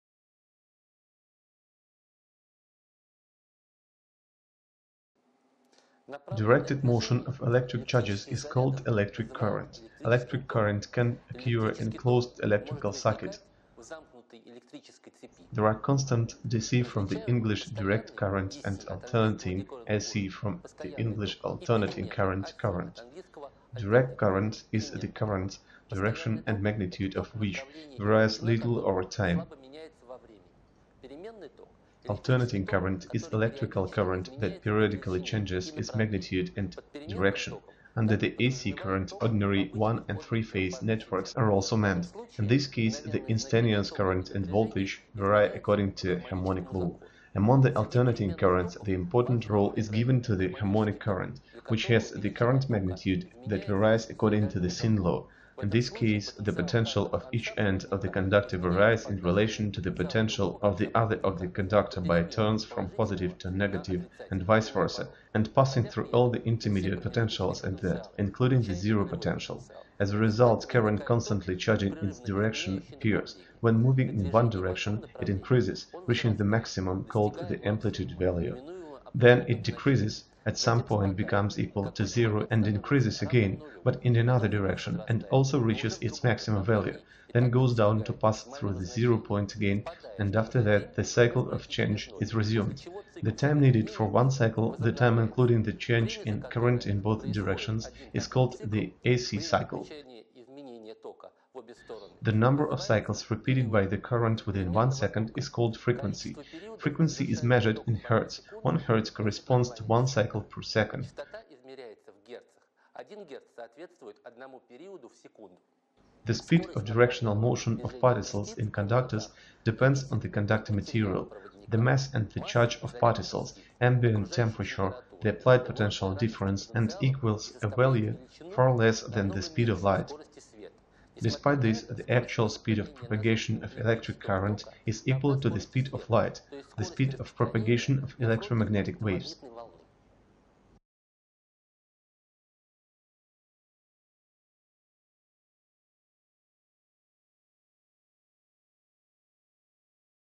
Types of electric current
Directed motion of electric charges is called electric current. Electric current can occur in a closed electrical circuit. There are constant (DC, from the English "Direct current") and alternating (AC, from the English "Alternating current") current.